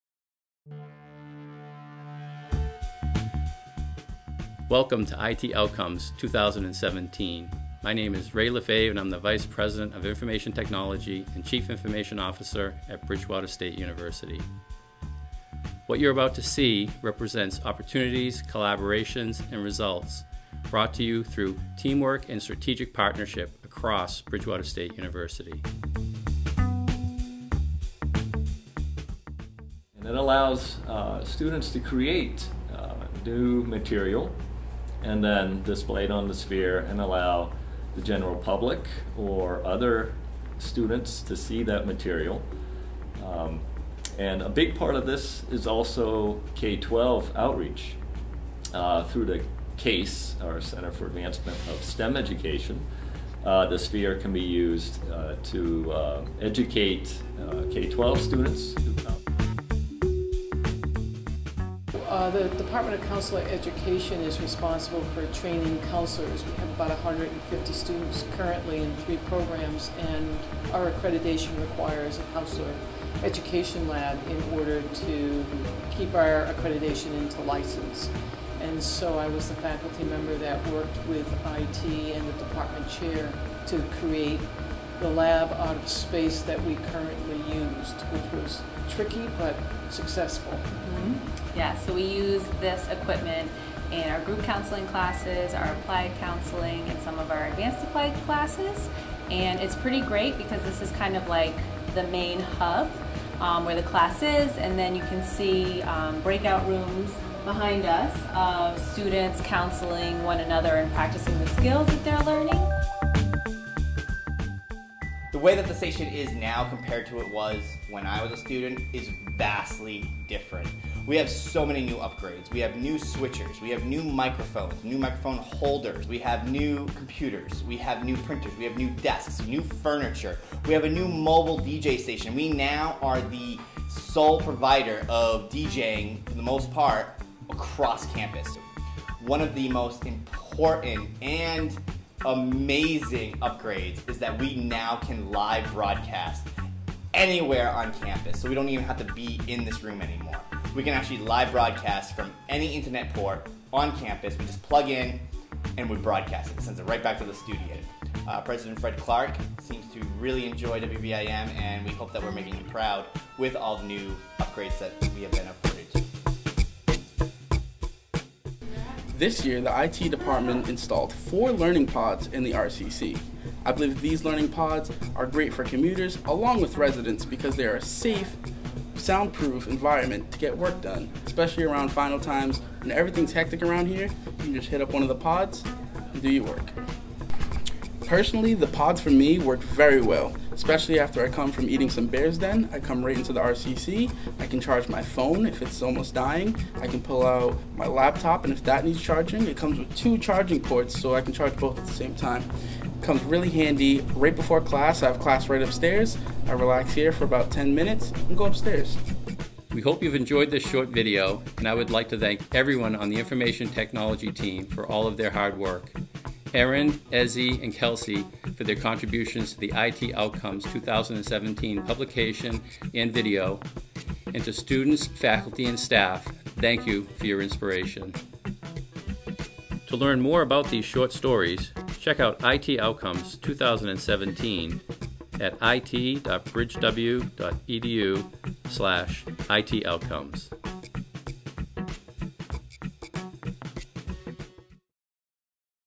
IT Outcomes 2017
The IT Outcomes 2017 video, and the companion publication located at it.bridgew.edu/ITOutcomes, represent inspirational stories of success at Bridgewater State University brought about through the collaborative efforts of the Information Technology team in strategic partnership with students, faculty, and staff.